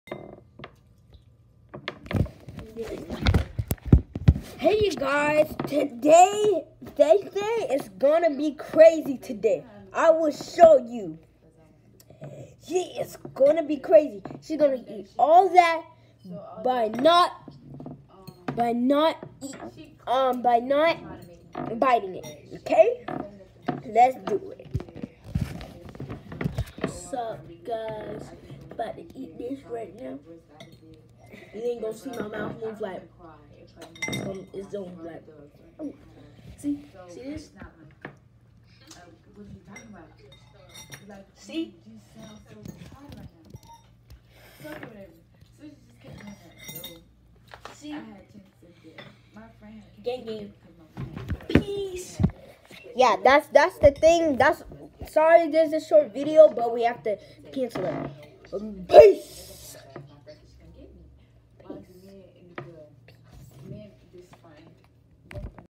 Eating noodles.
Eating noodles without biting it.